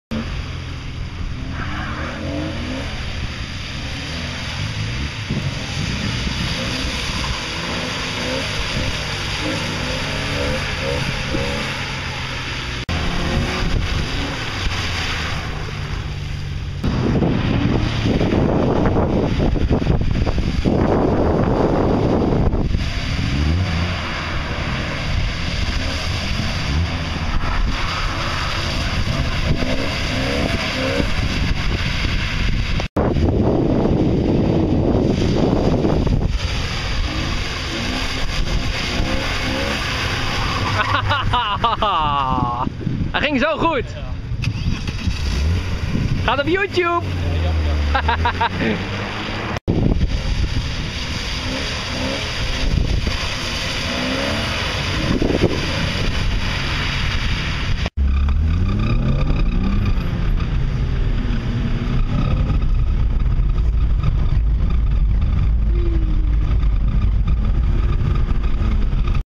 Walter Drift Cursus - Driftsport.nl
Begin cursus bijgewoond met mijn broer Walter.
Was uiteraard supergaaf en natuurlijk bij Driftsport.nl te Lelystad.